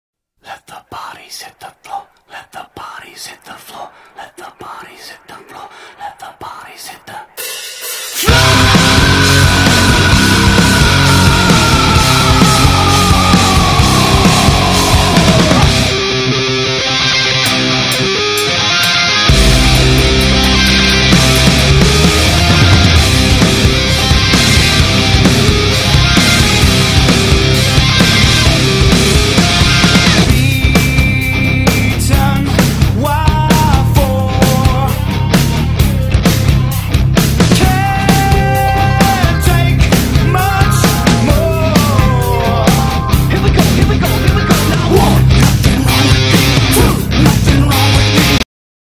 Custom Painted XBox Controller
*!*Made the stencils and painted Controller and wireless mic myself post comments and let me know if you want me to make a video about how to make your own stencils cheap and easy*!*